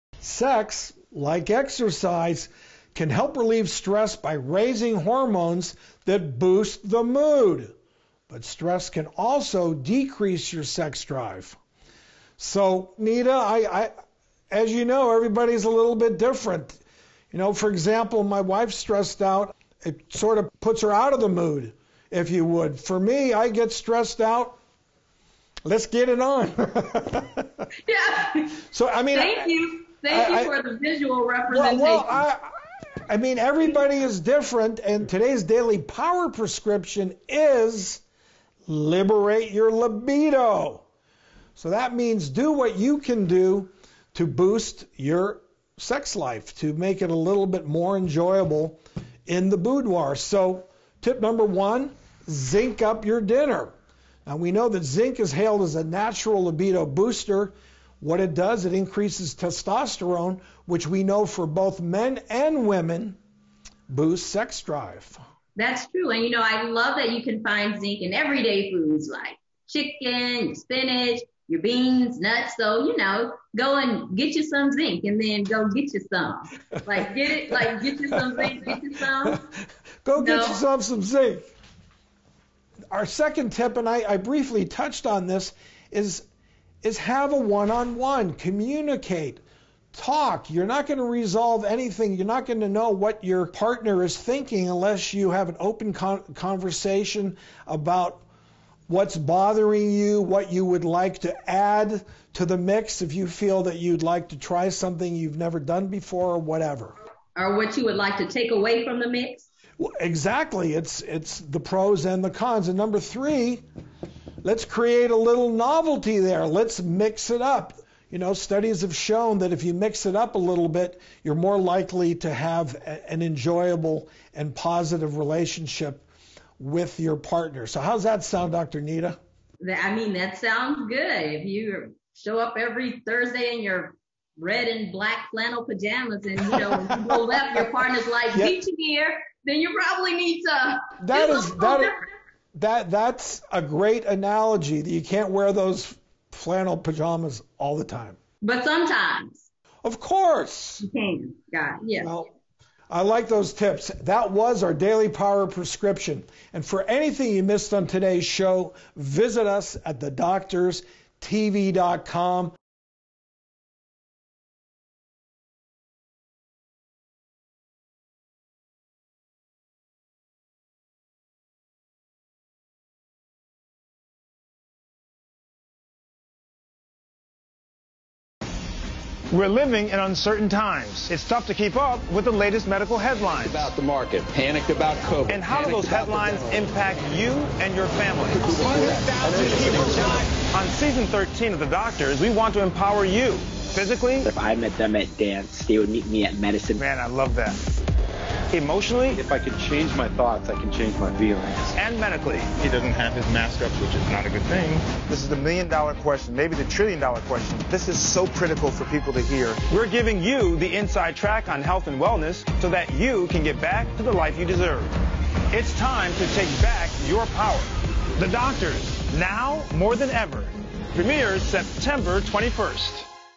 Liberate Your Libido!
Sex, like exercise, can help relieve stress. The Doctors share three tips to boost your libido.